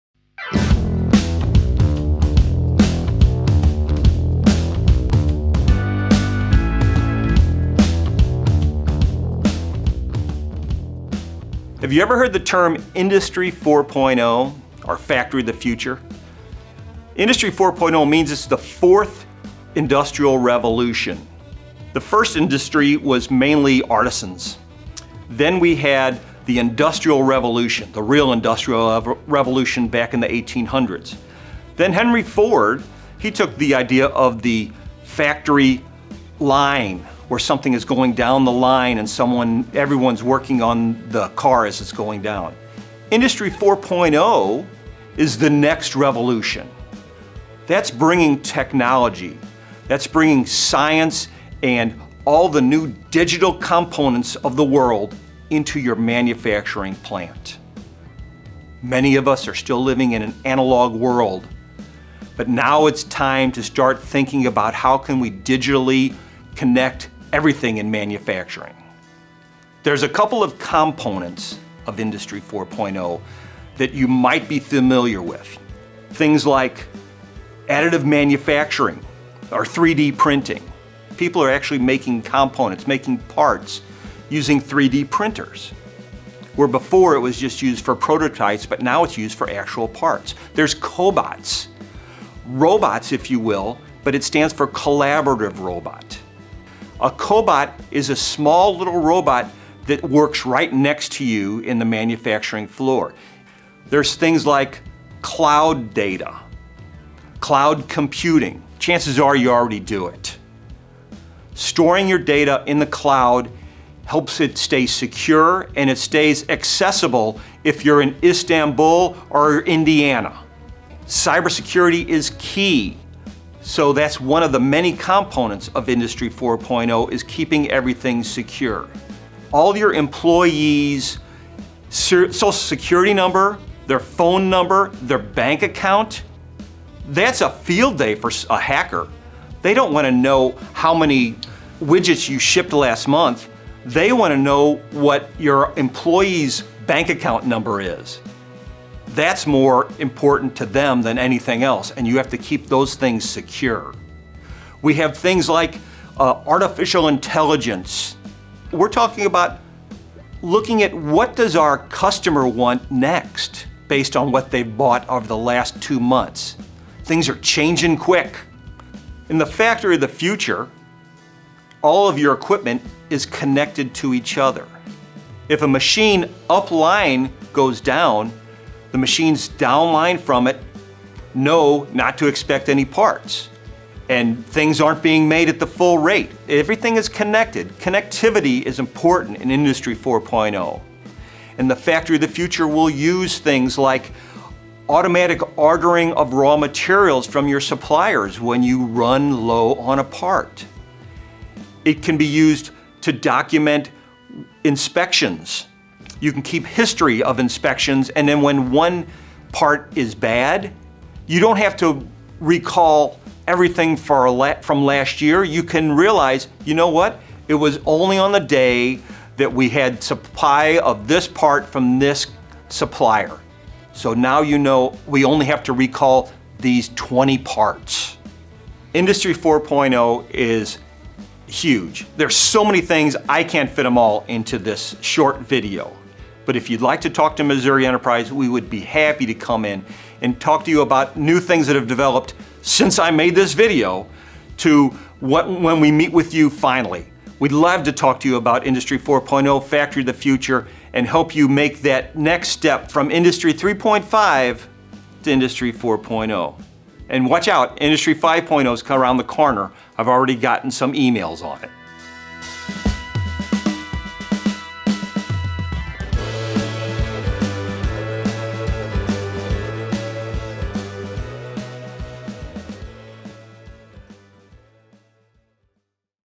Industry 4 Factory Future
Project Manager Bob Beckmann talk about Industry 4.0, Factory of the Future.